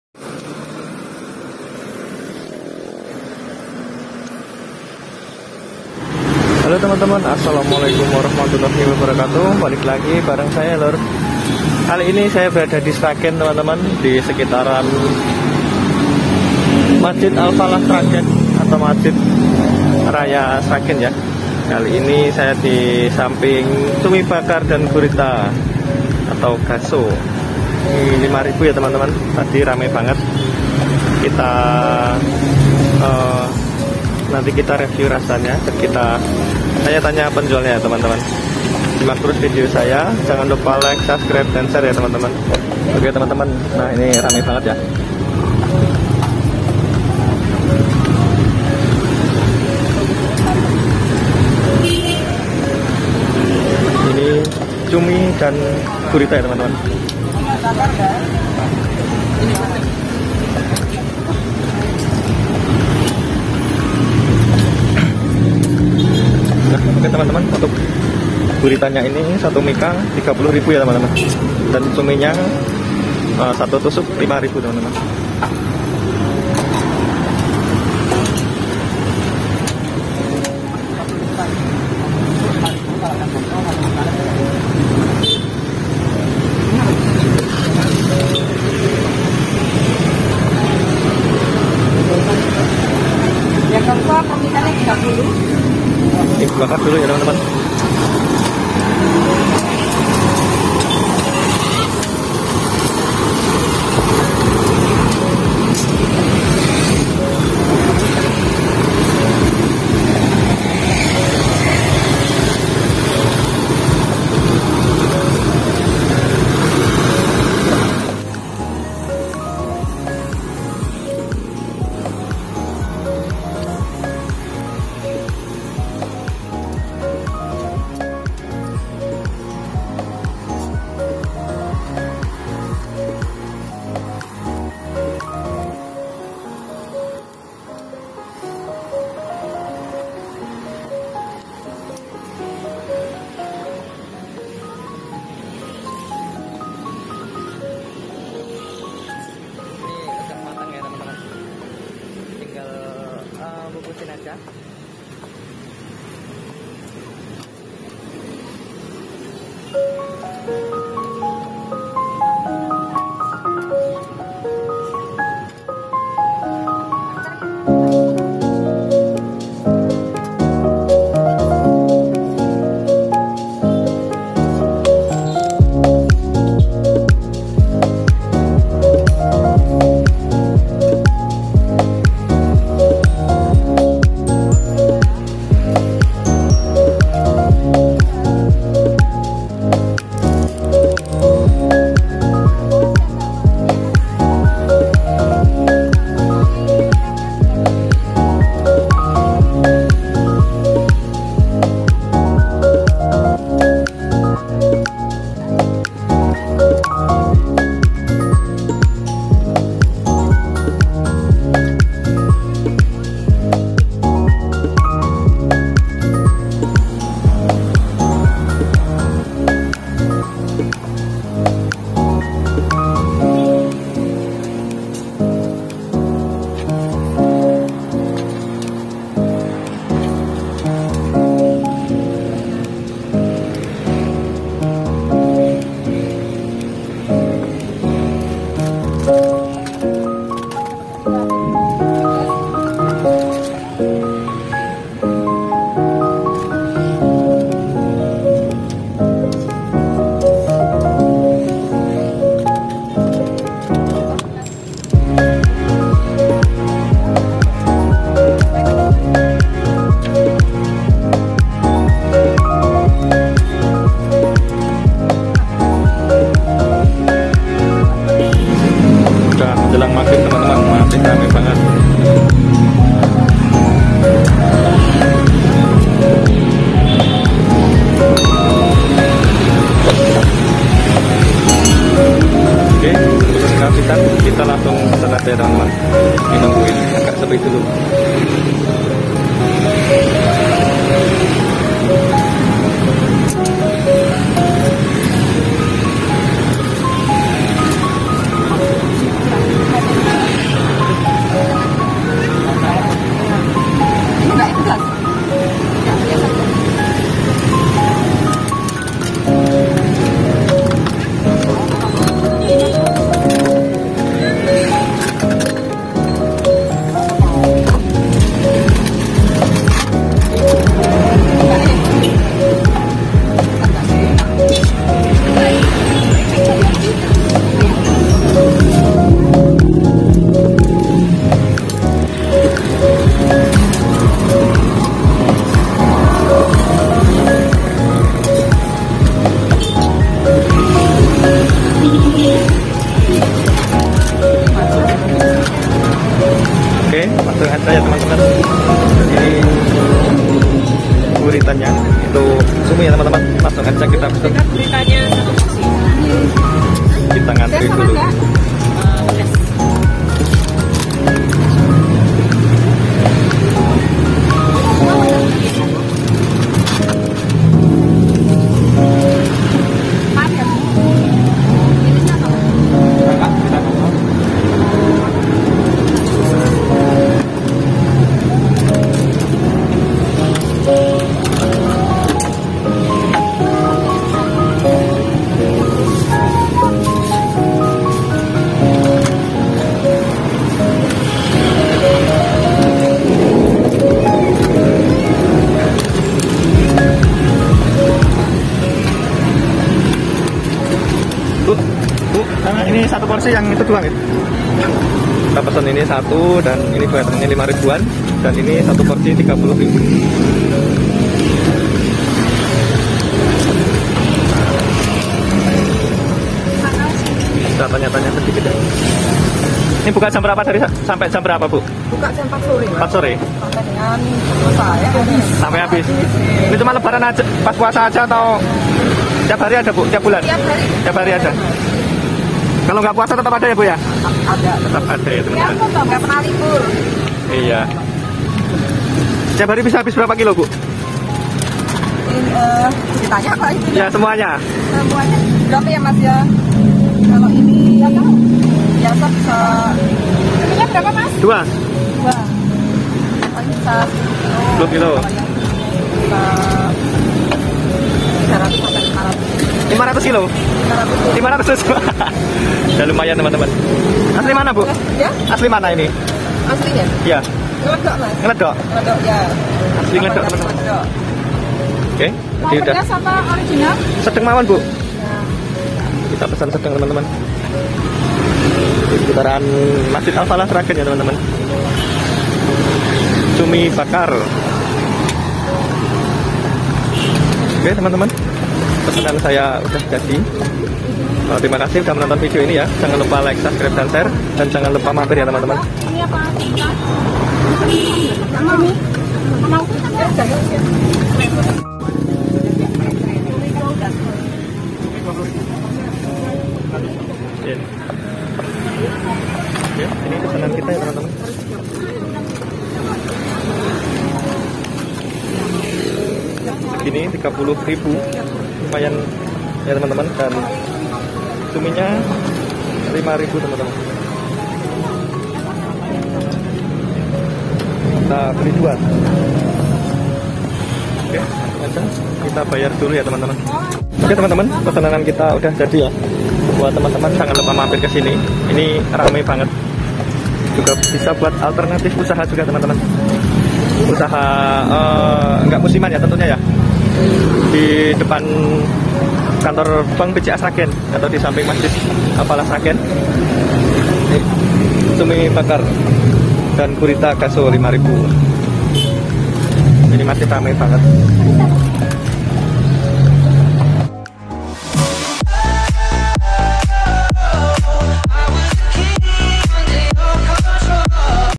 BARU BUKA LANGSUNG PADA ANTRI‼️ INSPIRASI USAHA ANTI MUSIMAN || CUMI & GURITA BAKAR
CUMI & GURITA BAKAR 5000
📍JL. sukowati (samping masjid al-falah sragen)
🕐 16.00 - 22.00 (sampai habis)
Cumi dan Gurita harga berbeda ya lur...